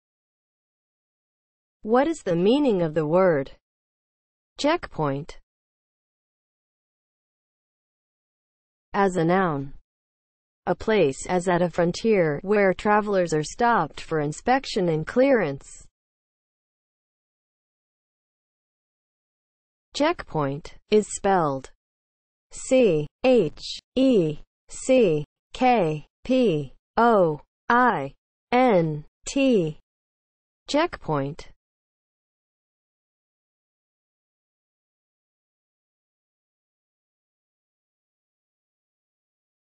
What is the meaning of the word CHECKPOINT?
Definition and spelling of the word CHECKPOINT.

(00:00) Intro
(00:08) As a noun
(00:19) Spelling

As a noun (00:08):
• a place (as at a frontier) where travellers are stopped for inspection and clearance